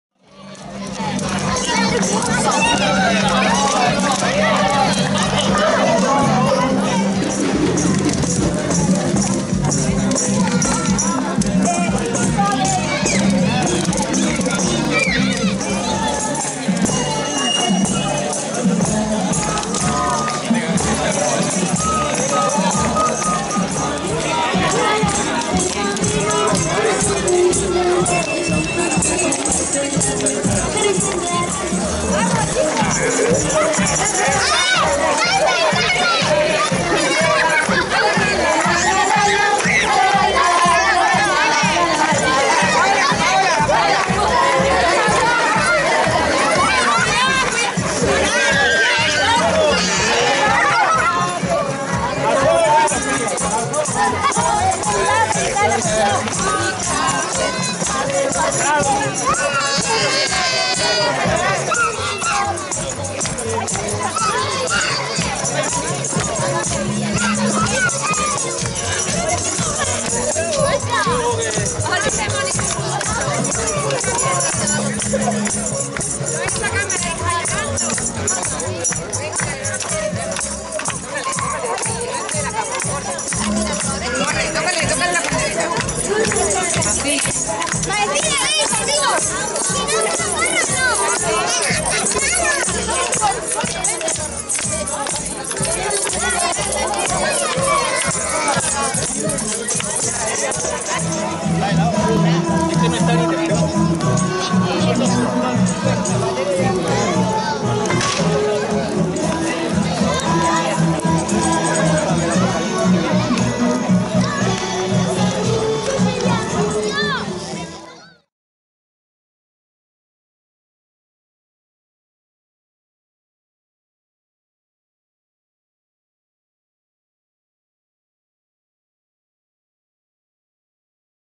Las ‘lumenarias’ calentaron la víspera de la Purísima en Abarán
En la noche del 7 de diciembre, víspera de la Inmaculada Concepción, es tradicional en Abarán celebrar una de nuestras costumbres más populares: las lumenarias (o 'lumbrenarias'), nombre con el que son conocidas en nuestro municipio estas tradicionales hogueras.